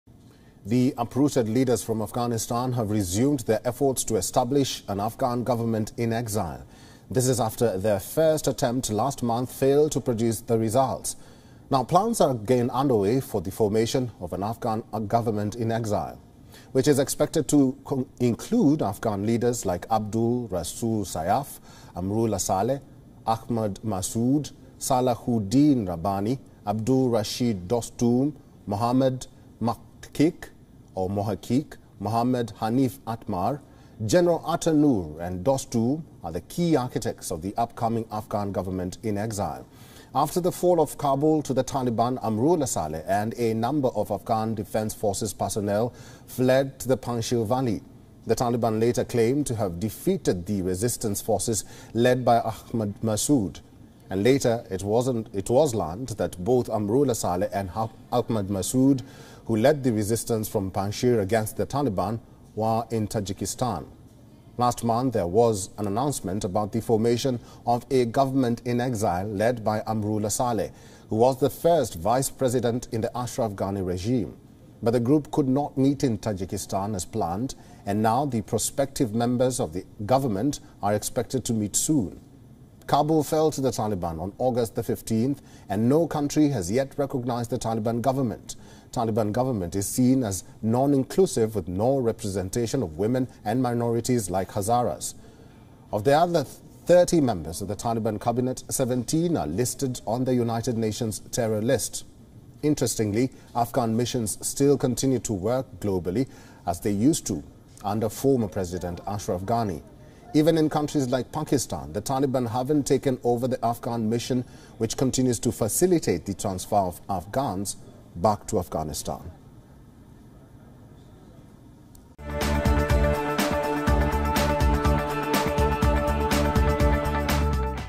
Uprooted leaders from Afghanistan resume efforts to establish an Afghan government in exile | WION
The uprooted leaders from Afghanistan have resumed their efforts to establish an Afghan government in exile, this is after their first attempt last month failed to produce the results. Now, plans are again underway for the formation of an Afghan government in exile.